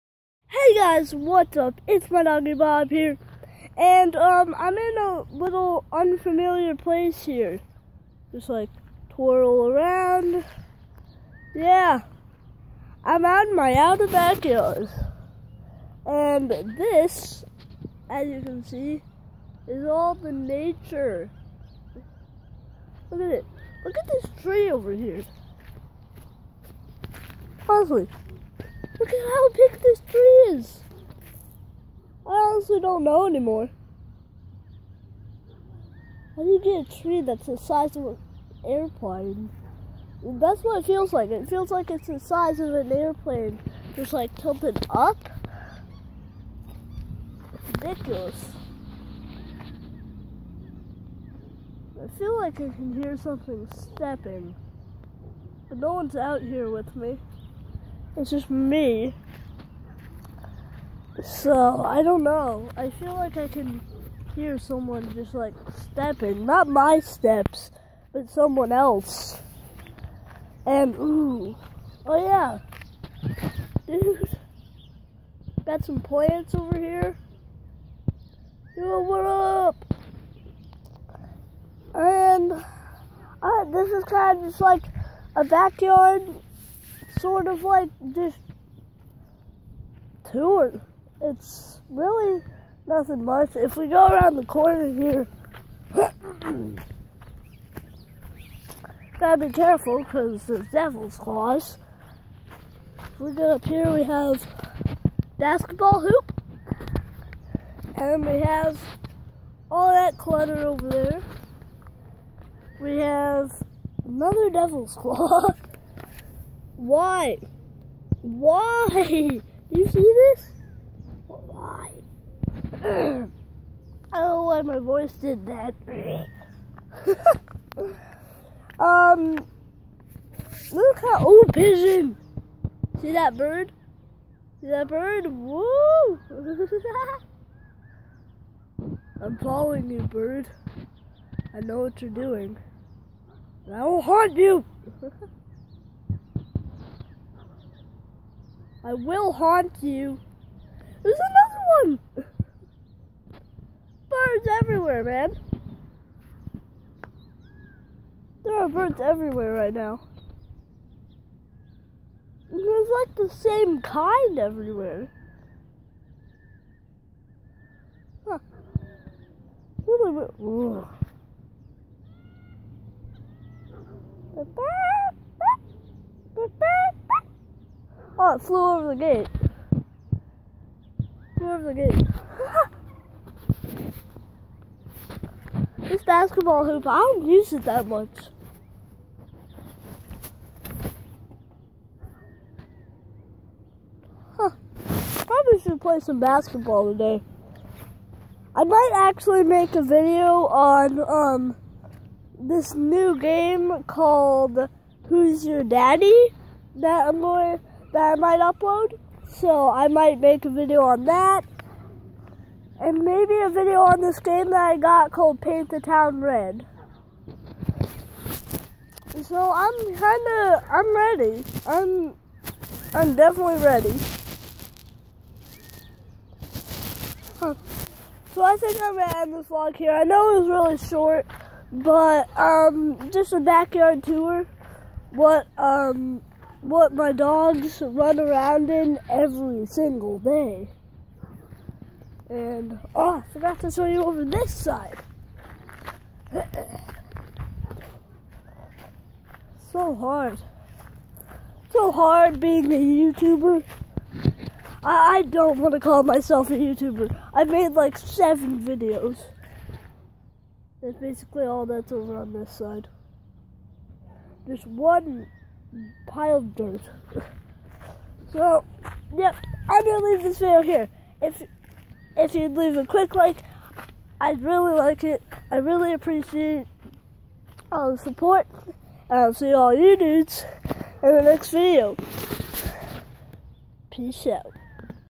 Vlog | BACKYARD TOUR???
Teagan shows you his nature and bird filled wonderland also known as his backyard!